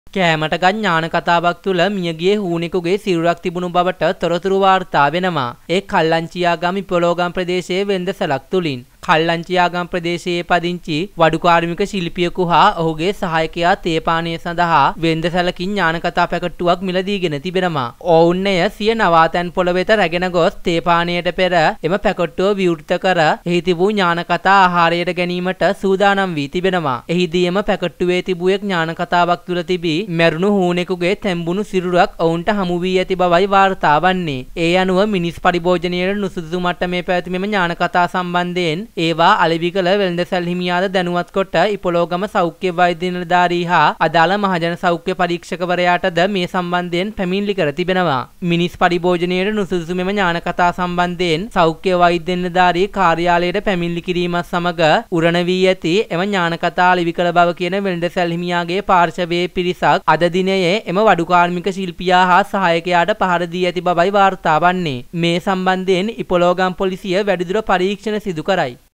ඥාණකතාවේ හිටපූ හූනා ගැන සාක්ෂි සහිත සම්පූර්ණ විස්තරය | What was in gnyanakatha
මෙම පුවත වාර්තාවන්නේ කල්ලංචියාගම ප්‍රදේශයේන්. මෙන්න විස්තරේ.

This news is reported from Kallanchiagama area. Here are the details.